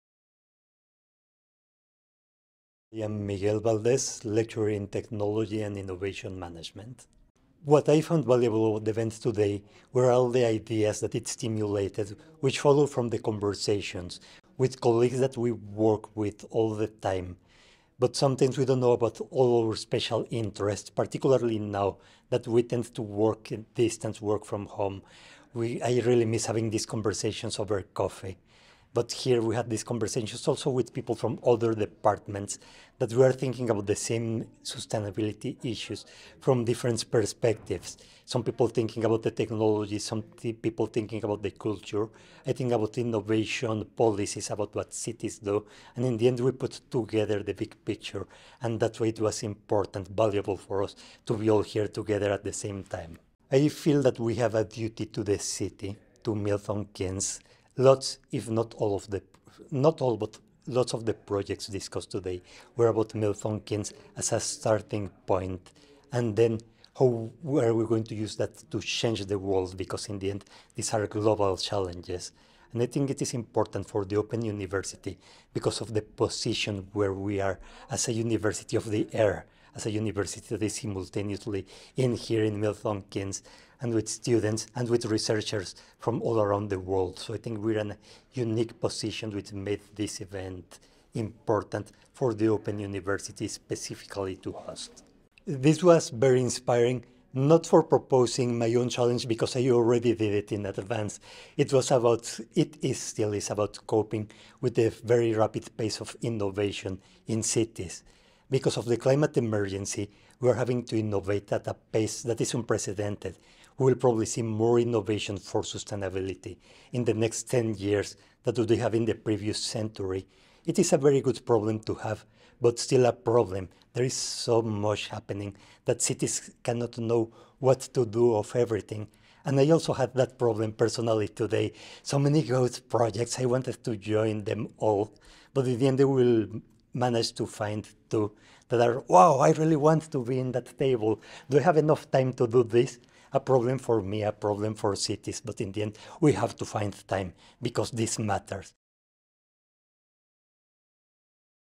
Why do we need to come together and collaborate in research? (Dr Miguel Valdez)
Miguel Valdez discussed why it is important to have the opportunity to come together and collaborate for research (particularly in Sustainability) and what the benefits of research events can be. Be it conversations over coffee, finding out about other’s perspectives and interests, or participating in a ‘sand pit’ session where you can start to think about pulling apart a complex issue in a different way. For more information on Research and The Open University, please visit our